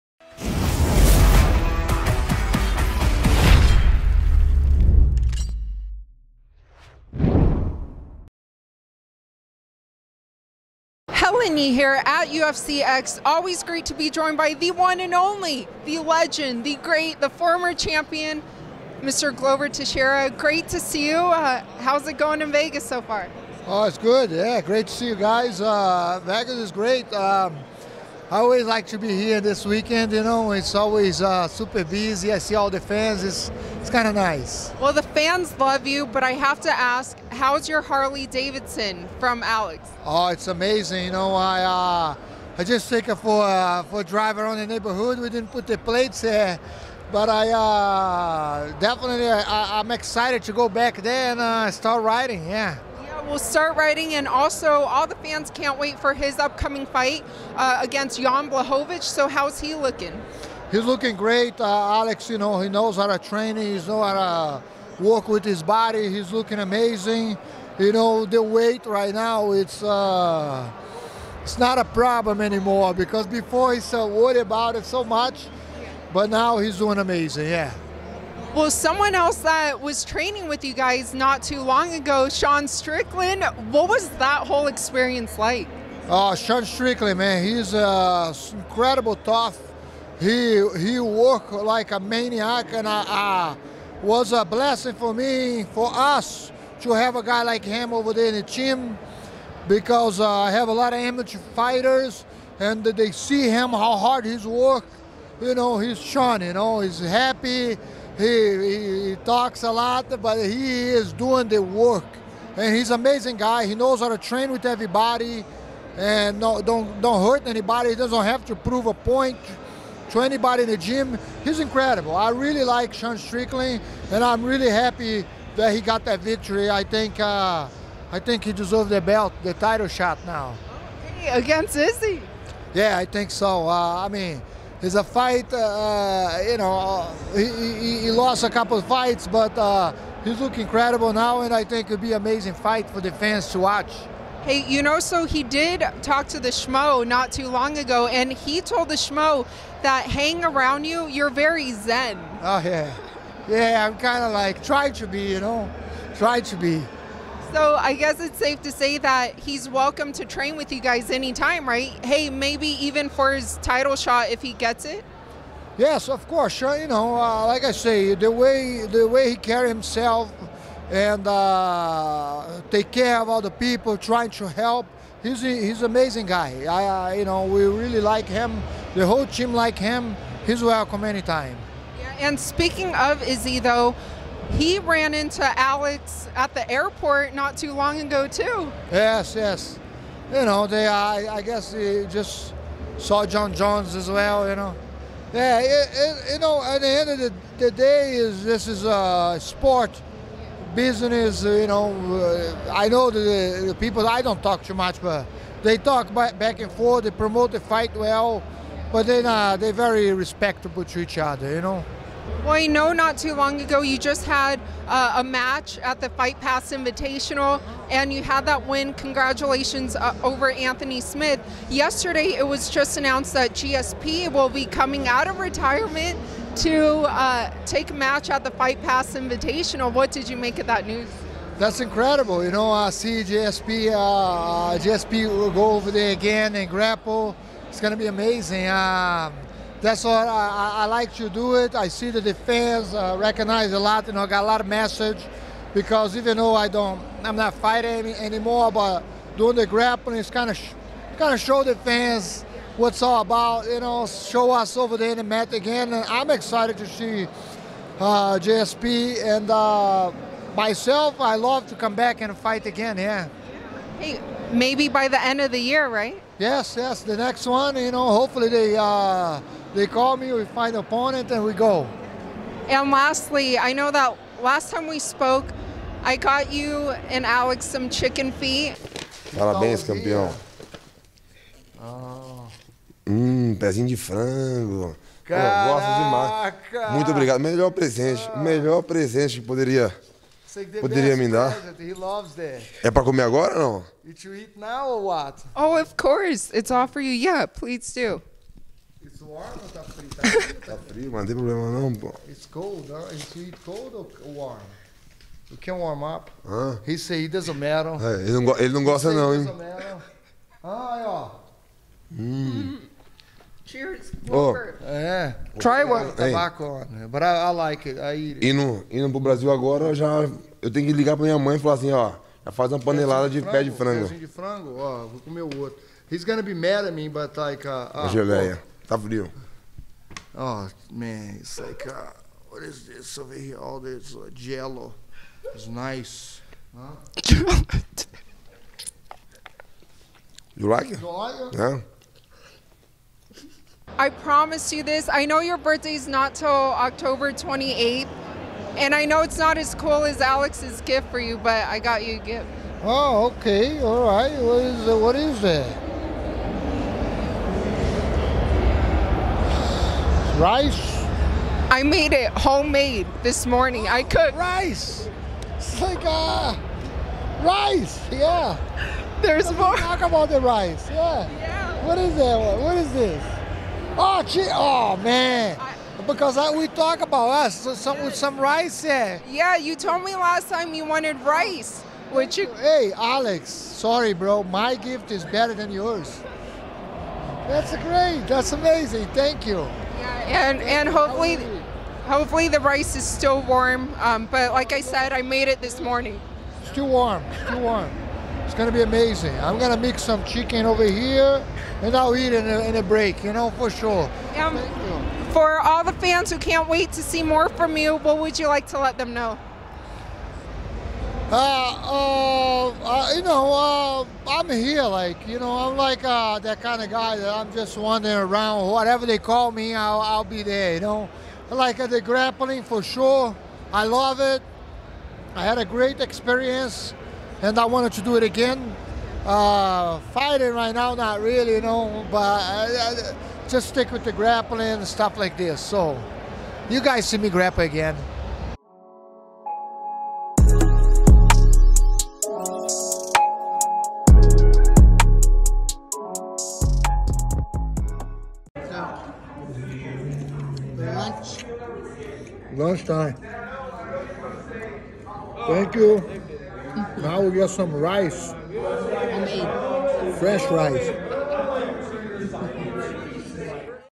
Glover Teixeira Details Training With Sean Strickland, Alex Pereira
At UFC X, Glover Teixeira spoke to Helen Yee about being in Las Vegas, riding his Harley Davidson which was a gift from Alex Pereira, Alex Pereira’s upcoming fight against Jan Blachowicz at UFC 291, Sean Strickland visiting and training at Glover Teixeira’s gym ‘Teixeira MMA and Fitness’, Israel Adesanya running into Alex Pereira and Jon Jones, Georges St-Pierre returning to grapple at UFC Fight Pass Invitational, his hopes to grapple again by the end of 2023, Helen's gift of chicken feet and rice, and more.
Outro Beats/Instrumental Music By: Akeem Muzik Beatz